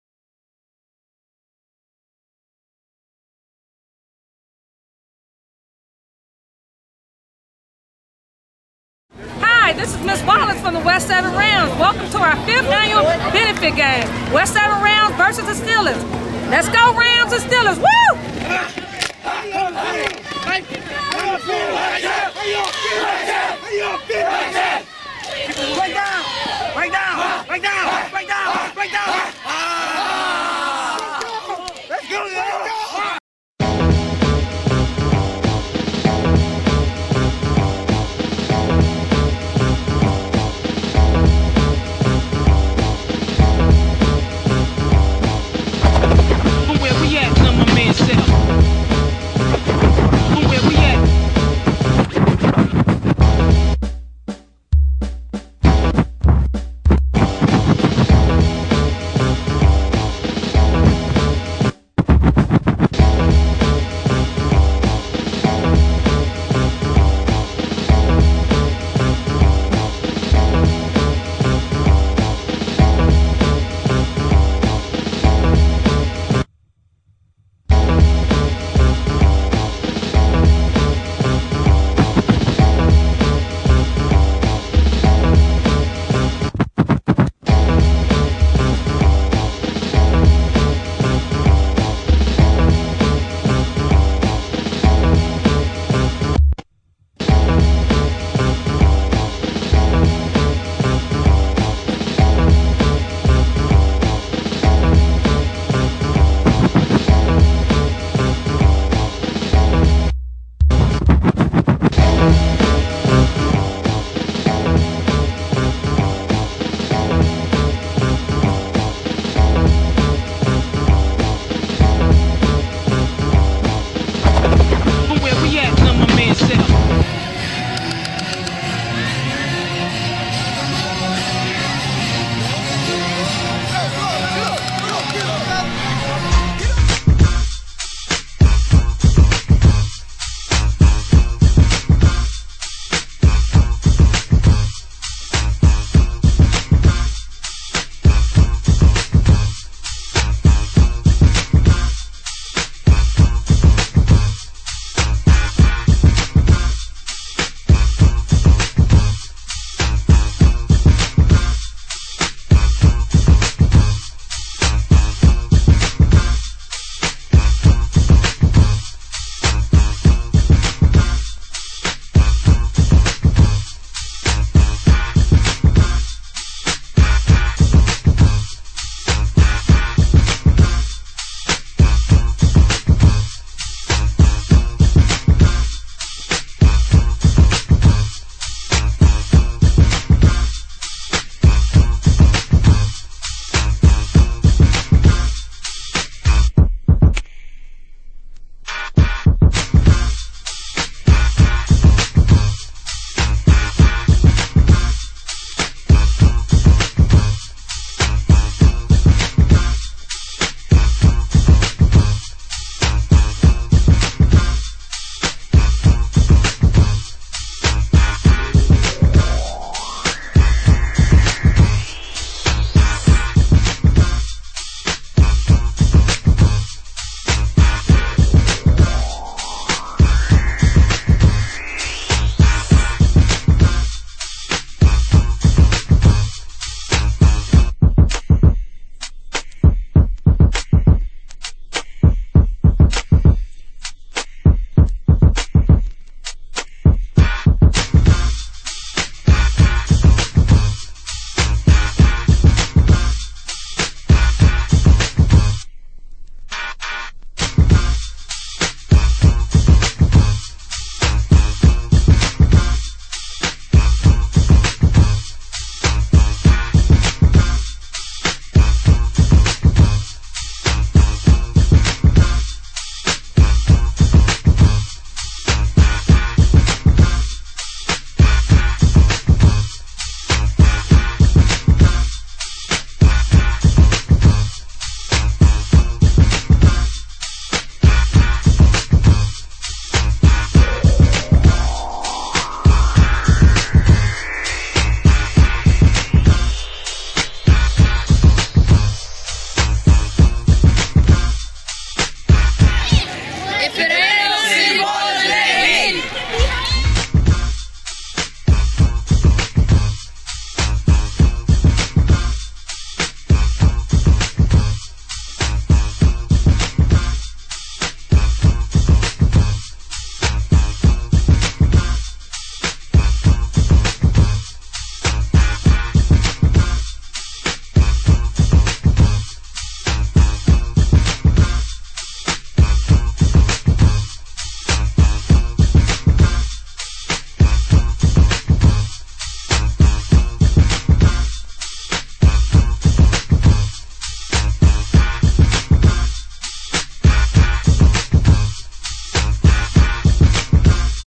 Detroit West 7 Rams vs Detroit Westside Steelers
(August 13th) 2011 Youth Benefit Game Detroit West 7 Rams vs Detroit Westside Steelers @ Detroit Renaissance High